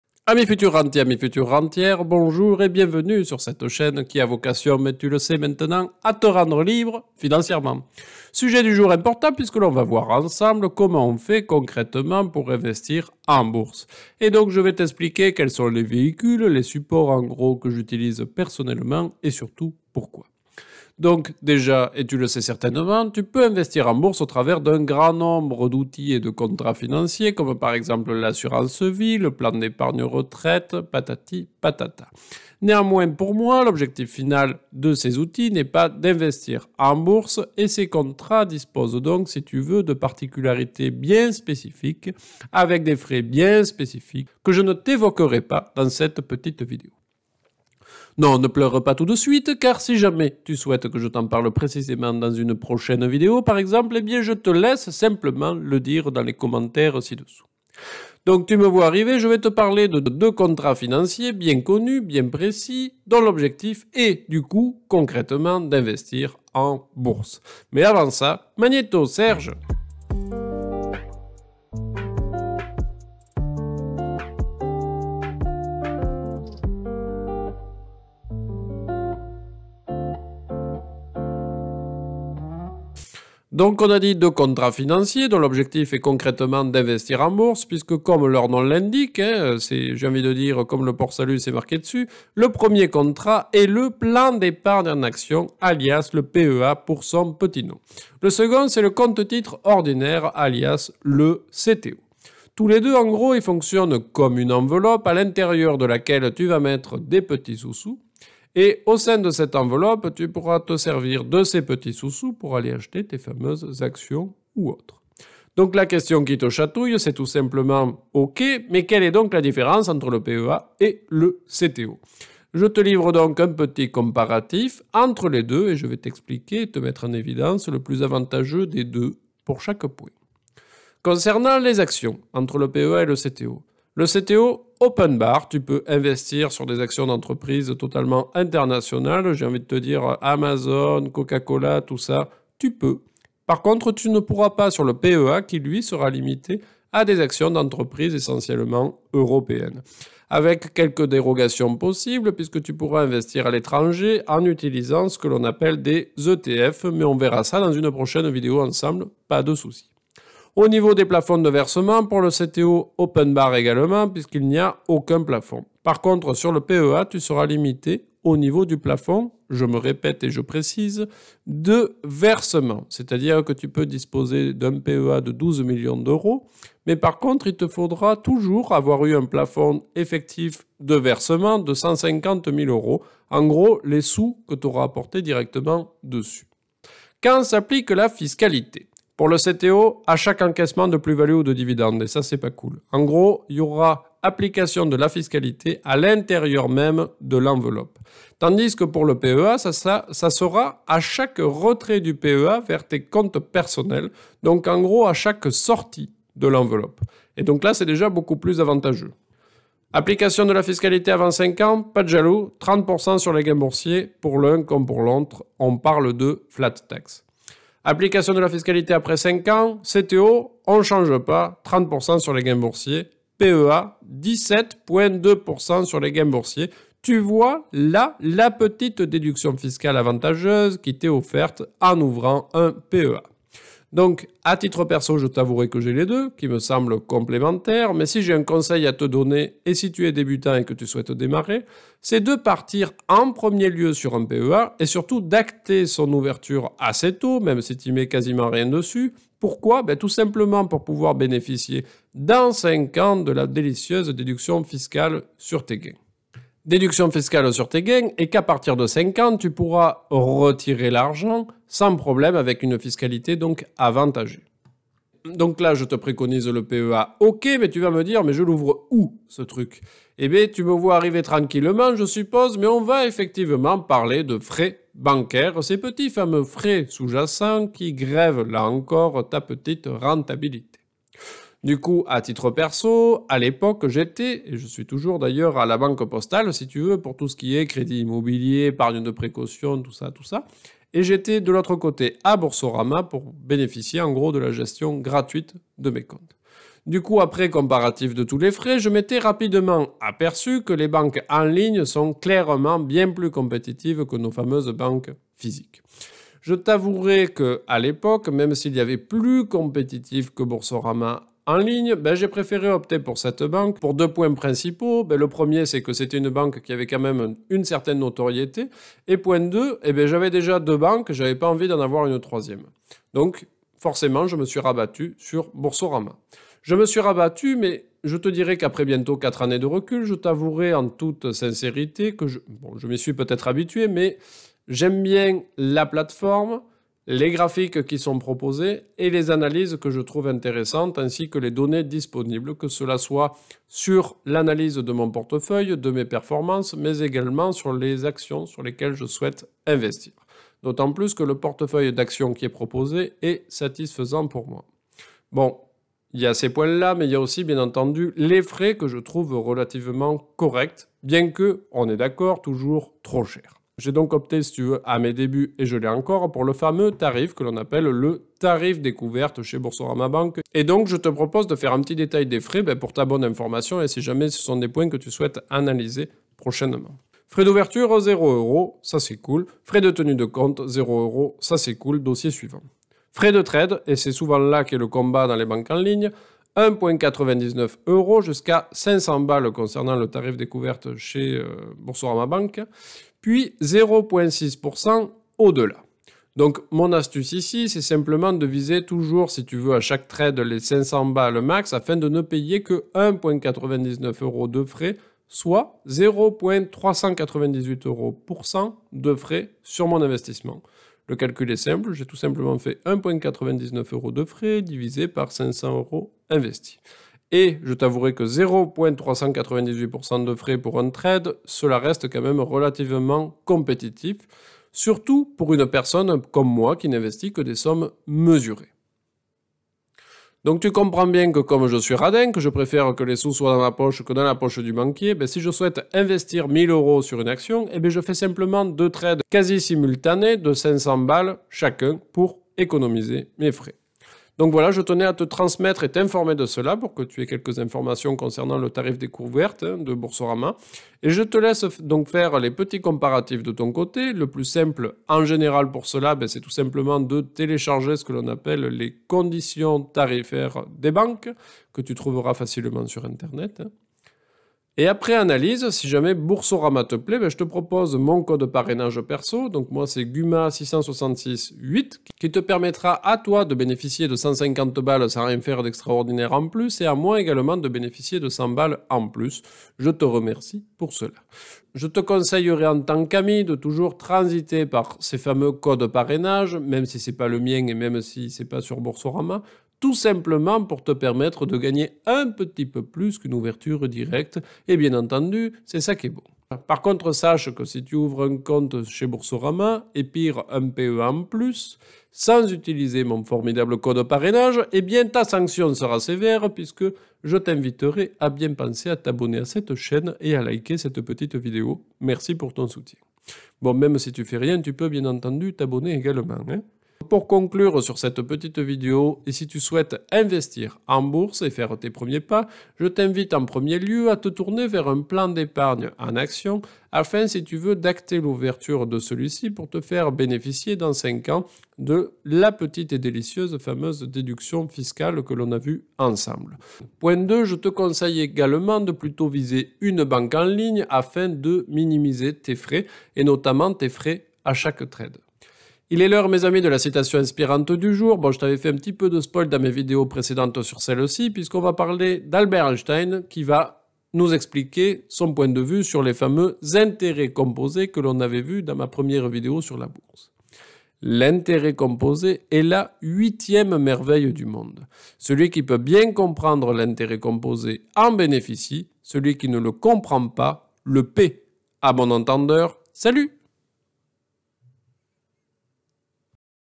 Comment INVESTIR en BOURSE 🤔? PEA ou CTO ? (débutant) - #19
Amis futurs rentiers, amies futures rentières, Salut à toi :)
#19 : Aujourd'hui, on commence à démarrer tranquillement en abordant la question du comment débuter l'investissement en Bourse ? En effet, quand on démarre et qu'on est débutant (comme ce fut mon cas il y a quelques années), on ne sait pas toujours quels outils utilisés, quels contrats et pourquoi ?
Afin de répondre à cette question simple, je te propose cette petite vidéo où je te montre les 2 contrats incontournables pour investir en Bourse à savoir :
*Le Plan d'Epargne en Action ou PEA
*Le Compte Titre Ordinaire ou CTO
On verra ensemble un petit comparatif de ces 2 contrats et surtout pourquoi je te préconise clairement le PEA en premier  si tu es débutant :).

Bon investissement à toi et bon visionnage !

PS : pour conseil, analyse ou autre, n'hésites pas à , par mp sur mon Facebook ou encore directement sur mon site (section "A propos" - "Liens" de cette chaîne)

 *LES LIENS INTERESSANTS :
Ma bible (le bouquin "Devenez Riche" de Rami SETHI")

Objectif Rentier :)

"00:00" Introduction à cette délicieuse Vidéo ;)
"01:09" L' incroyable générique !
"01:23" Les 2 incontournables pour Investir en Bourse !
"01:59" Comparatif entre le PEA et le CTO !
"04:34" Les Frais Bancaires de mon PEA Boursorama
"07:49" Mon Code Parrainage si Boursorama te plait
"08:17" La sanction ;)
"08:37" En conclusion... ;)
"09:07" Citation d'Albert EINSTEIN sur les Intérêts composés !